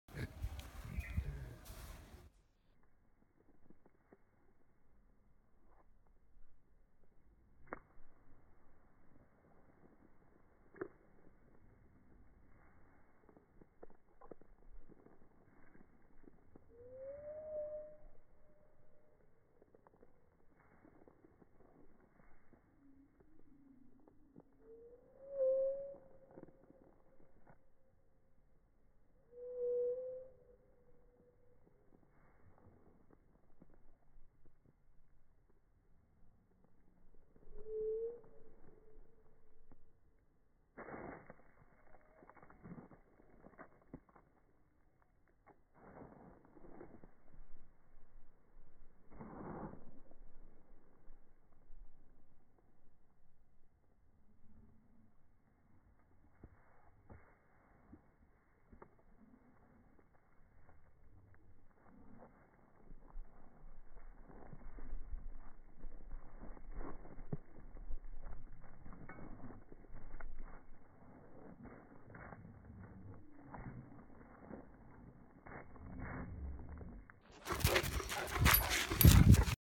Dollar (St. Bernard)
Slow motion of St. Bernard "Dollar"